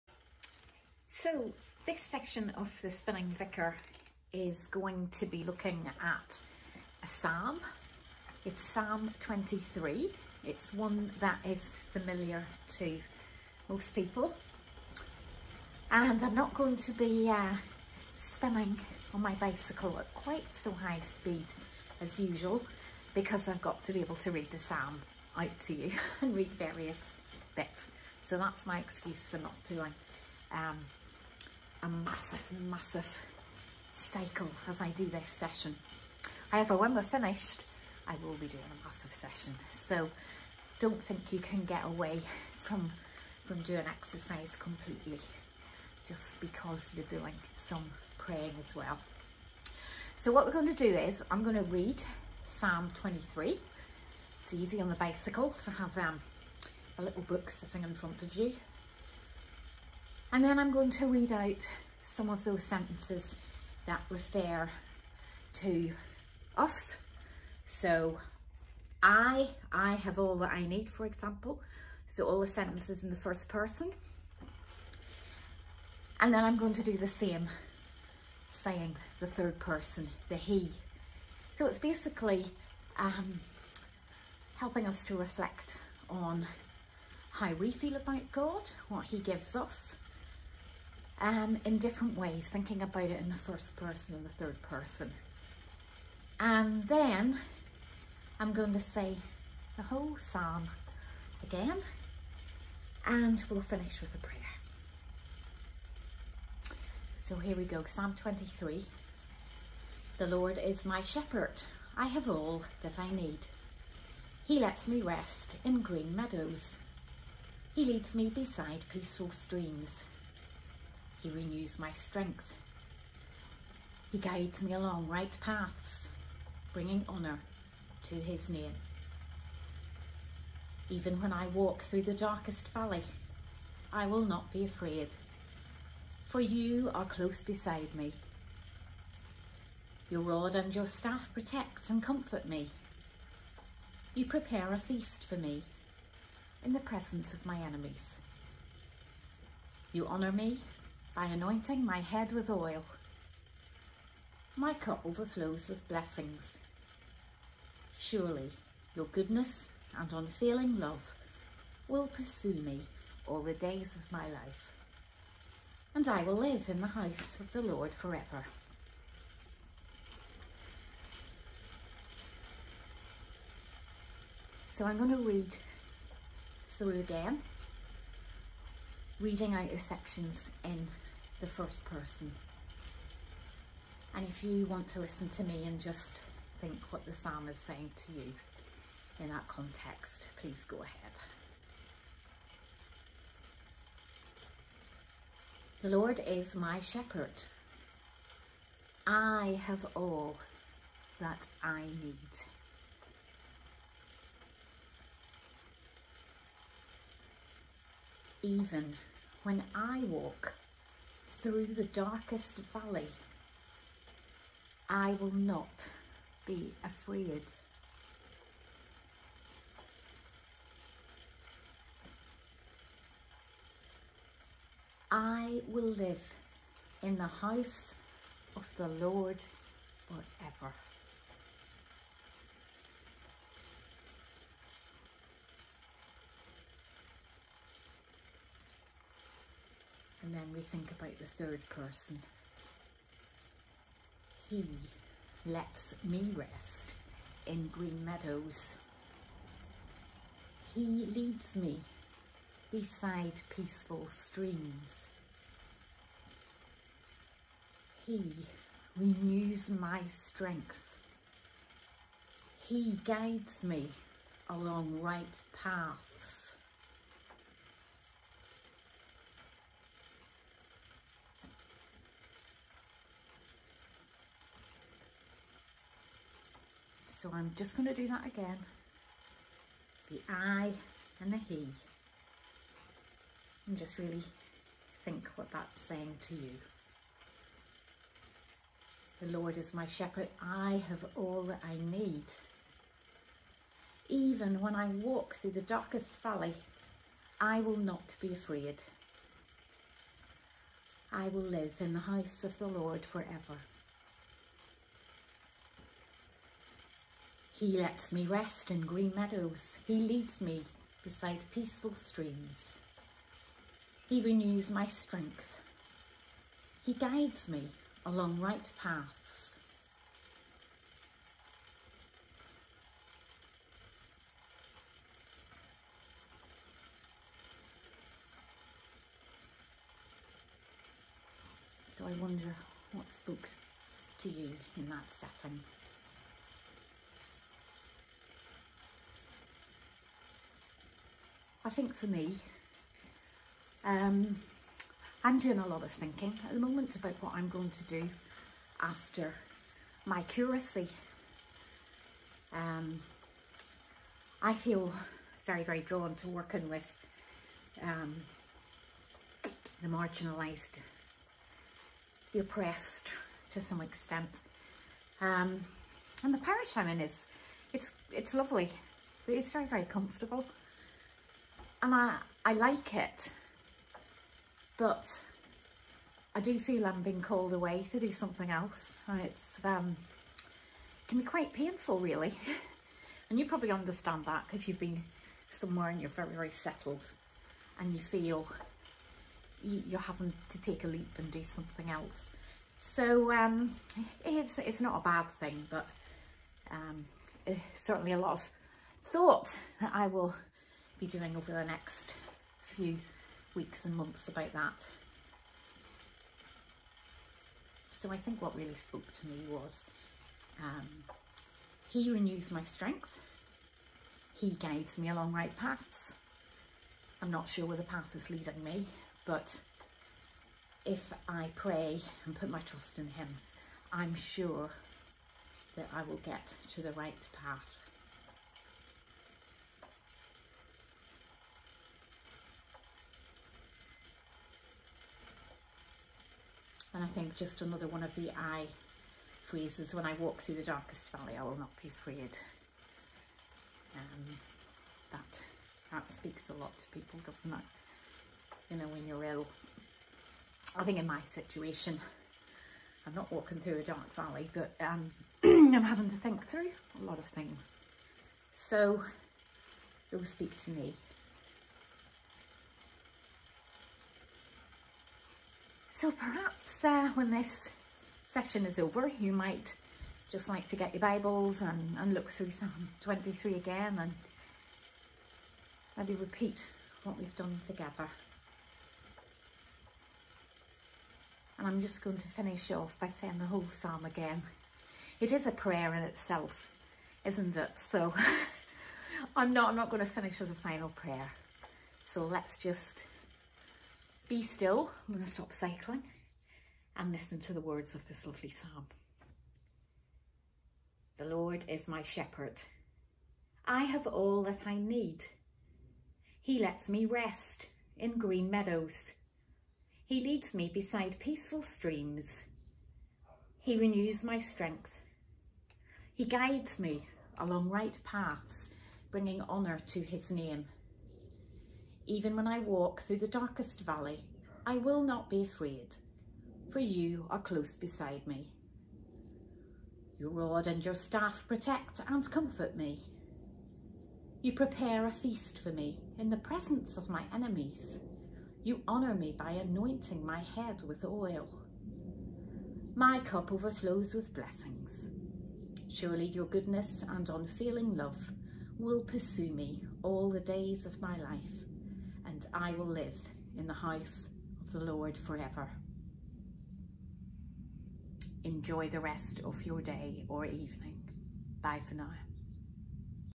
The Spinning Vicar: Session 6 🚲
In this Spinning session, I'm looking at Psalm 23. 🙏